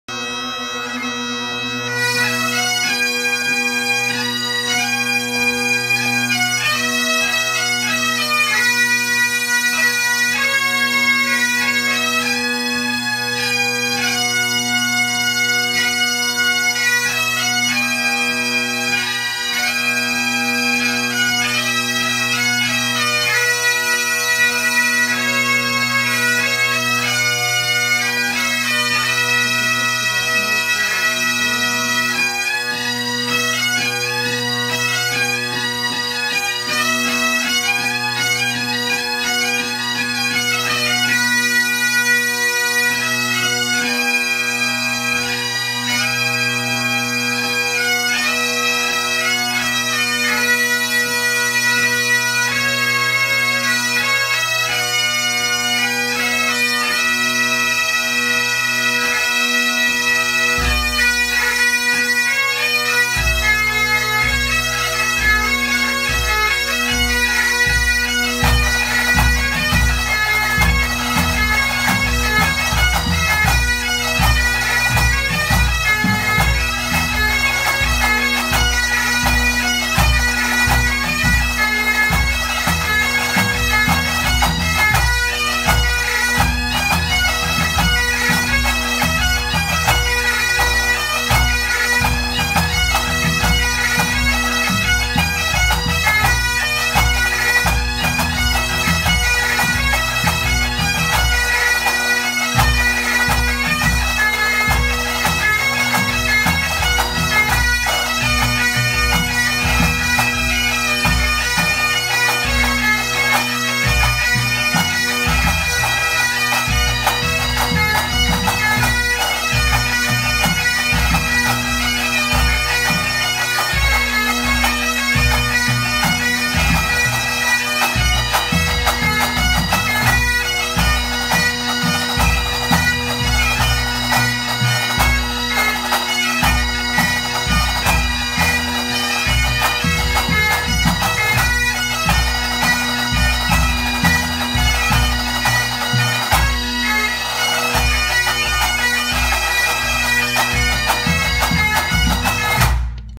RNZPBA Foundation Youth Band Pipe Band at Summer School - 3
Royal New Zealand Pipe Band Association Foundation Youth Band performing at the 2016 RNZPBA Summer School